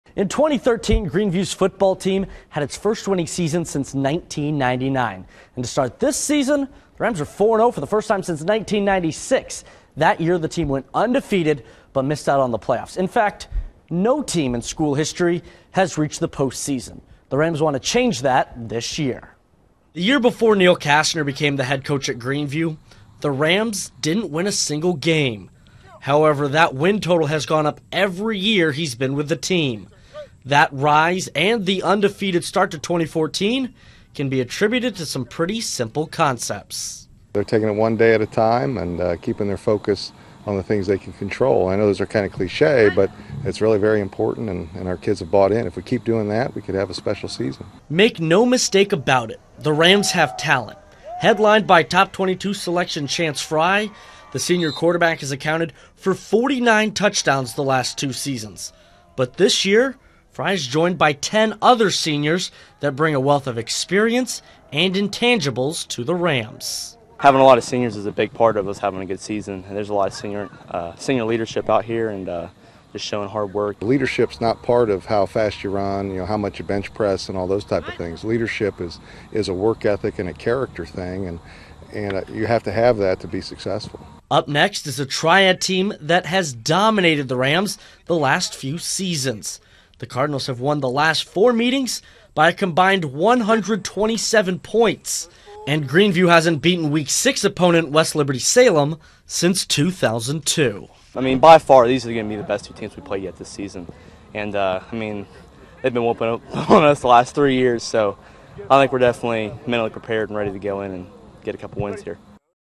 Greeneview 4-0 and Hoping to Make History, B
JAMESTOWN -- In 2013 greeneview's football team had its first winning season since 1999. To start this season the Rams are 4-0 for the first time since 1996.
   That year the team went undefeated but missed out on the playoffs. In fact no team in school history has reached the postseason.
   The Rams want to change that this year.
   The year before Neal Kasner became the head coach at Greeenview the Rams didn't win a single game. That win total has gone up every year he's been with the team. That rise and undefeated start to 2014 can be attributed to some pretty simple concepts.
   "They're taking it one day at a time and keeping their focus on the things that they can control," Coach Kasner said.
   "I know those are kind of cliche but it's really very important and our kids have bought in and if we keep doing that we could have a special season."
   Make no mistake about it, the rams have talent headlined by top 22 selection Chance Frye, the senior quarterback has accounted for 49 touchdowns the last two seasons. But this year Frye is joined by 10 other seniors that bring a wealth of experience and intangibles to the rams.
Adrian harding
((Switch))
   "Having a lot of seniors is a big part of us having a good season," senior linebacker and tackle Adrian Hardin said.
   "There's a lot of senior leadership out here and just showing hard work."
   Kasner added, "leadership's not part of how fast you run or how much you bench press and all those type of things. Leadership is a work ethic and character thing.  And you have to have that to be successful."
   But now Greeneview enters the teeth of its schedule. Up next is a Triad team that has dominated the Rams the last few seasons. The Cardinals have won the last four meetings by a combined 127 points. And Greeneview hasn't beaten week 6 foe West Liberty Salem since 2002.
   "I mean by far these are going to be the best two teams we've played yet this season," Hardin said. "They've been whooping up on us for the last three years so I think we're definitely mentally prepared and ready to go in and get a couple wins here."
   Even with a couple big wins don't expect Kasner to let the team lose focus. After all it's simple cliches that have Greeneview where they are. Four years removed from an 0-9 season the Rams are eying the program's first ever trip to the playoffs.